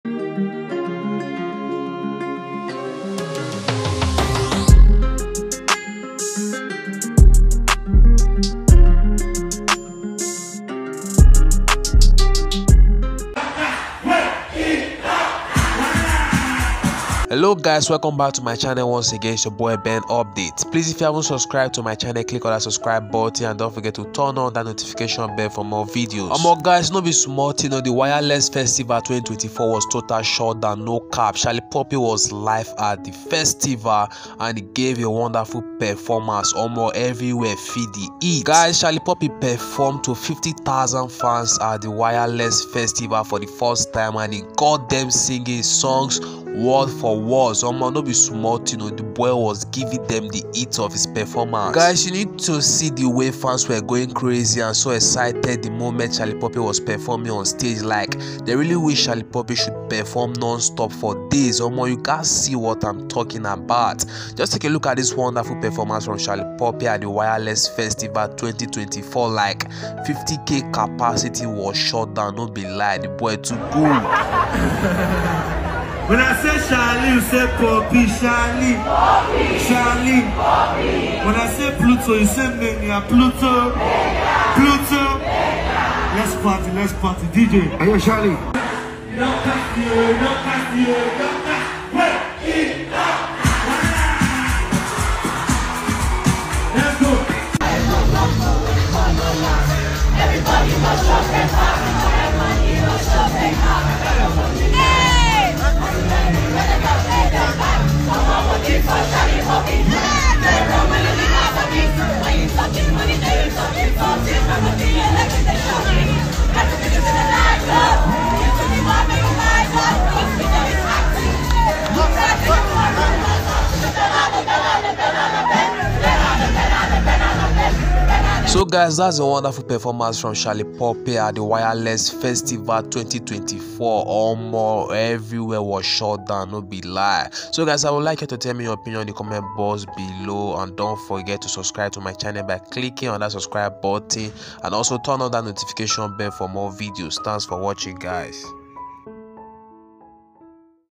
Shallipopi Shøck Wizkid And Davido As He Shutdown 50K Capacity Concert LIVE In London!!
Shallipopi performed to 50,000 fans at the Wireless Festival for the first time and he got them singing his songs words for words!!